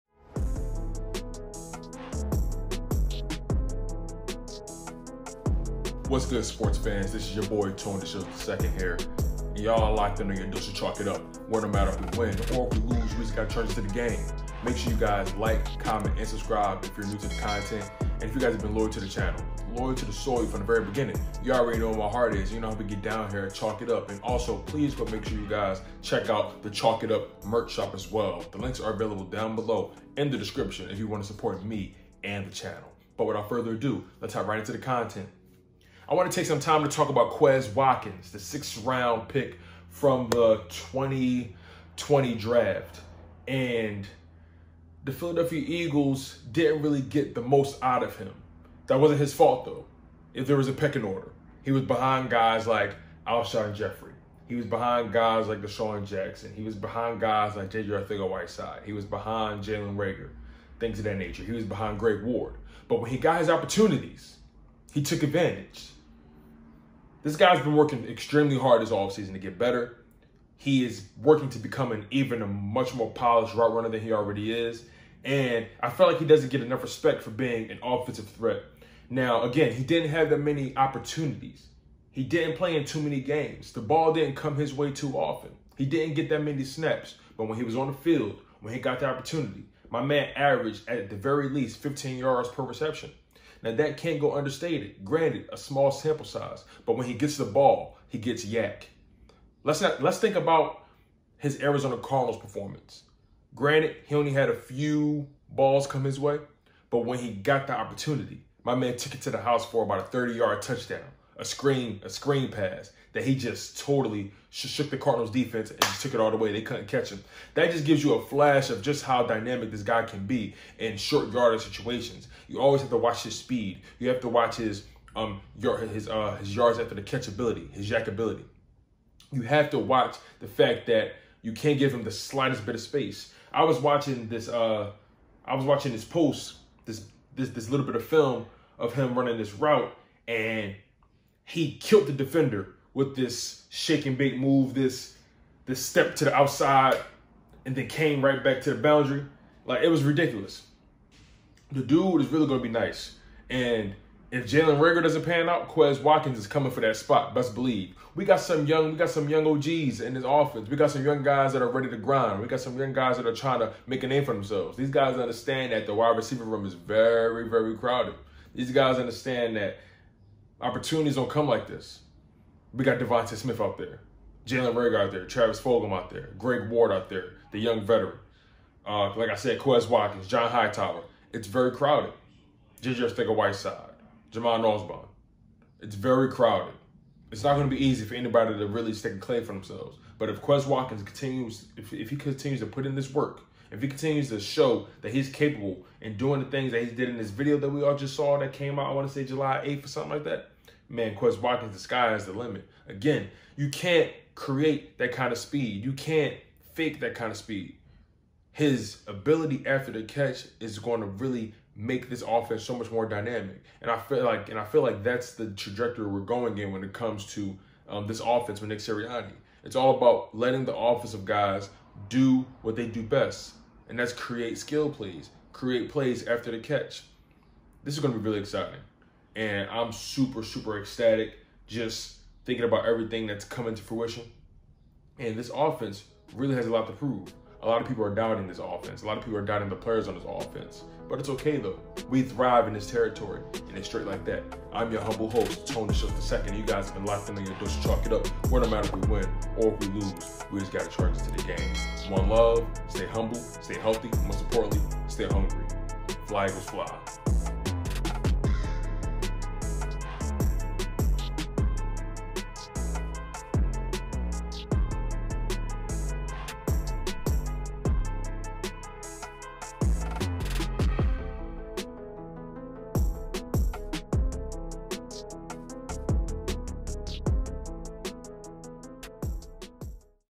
QUEZ WATKINS IS THE BEST KEPT SECRET | IS QUEZ WATKINS BETTER THAN JALEN REAGOR?
Quez Watkins has been having one hell of an offseason and he's making sure he gives himself every opportunity to make it in the NFL. Quez came in the league as a 6th round pick who did not have many opportunities to shine, but when the time came, he flashed like a solar flare, taking his second career catch to the house in Arizona Cardinals territory. Can Quez Watkins take his skillset to the next level, and is his upside beyond Jalen Reagor's?

*WANT TO SUPPORT TONE DESHIELDS II?*